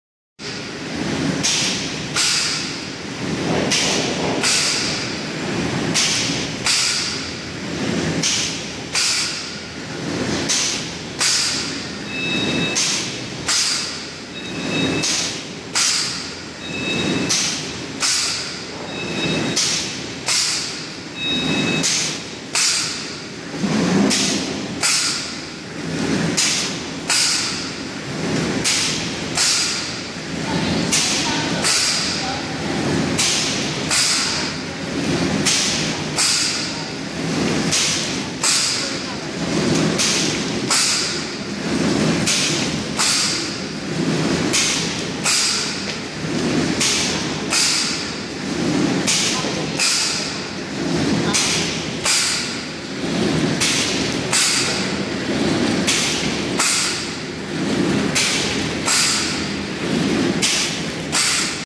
Plastic Bag Making Machine
We provide bag making machines with servo motors and their controls, as well as mechanical clutches and breaking systems according to customer requirements. It can process and produce packaging bags of various sizes and thicknesses of plastic or other materials, mainly plastic packaging bags.